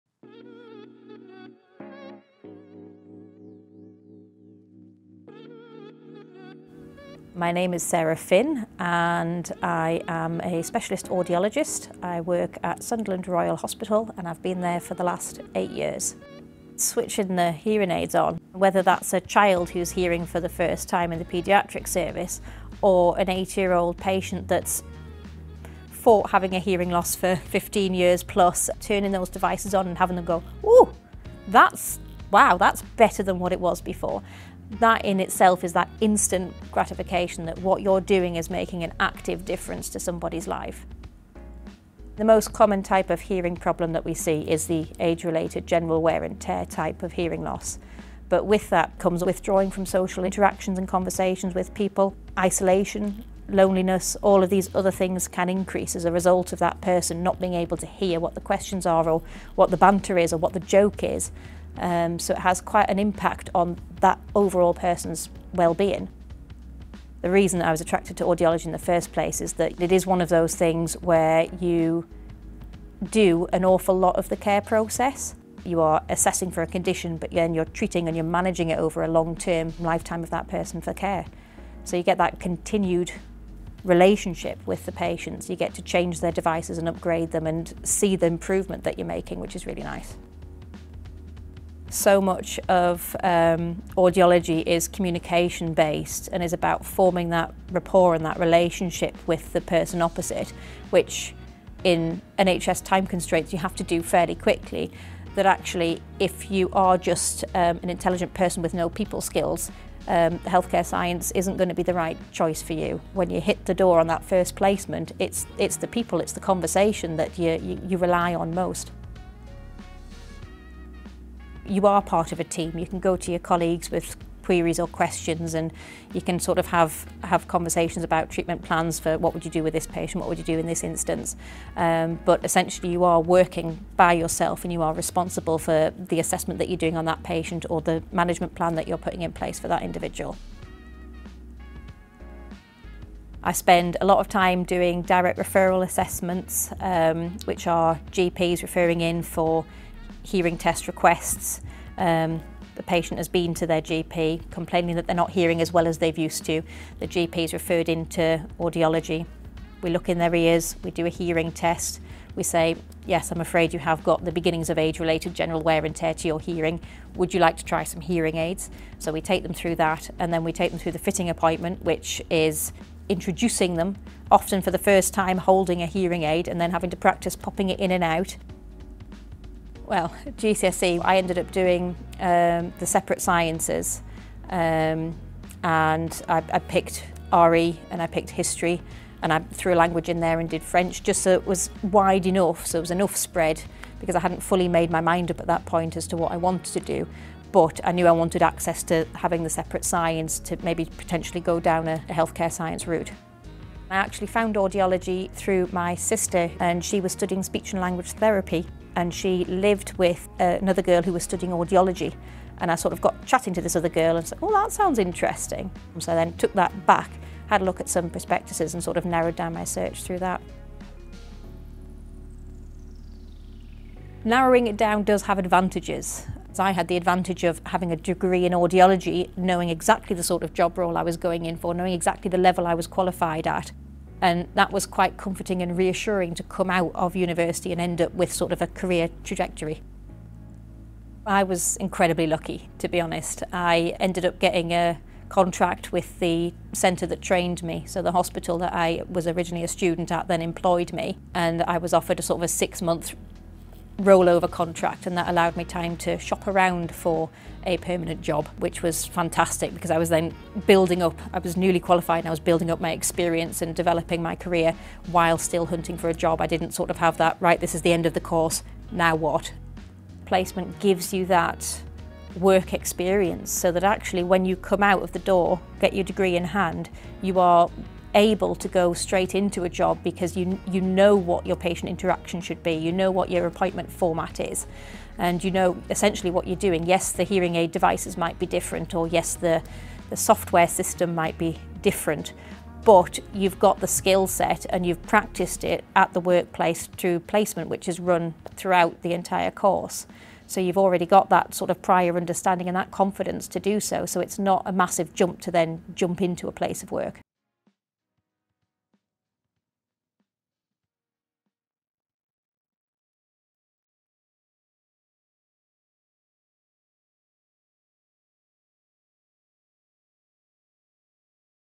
My Career as a Specialist Audiologist - Sarah's Story
A short video featuring Sarah, explaining her career as a Specialist Audiologist.

The video was produced by the Health & Science Careers Hub North East to help explain this exciting job role in the health science sector.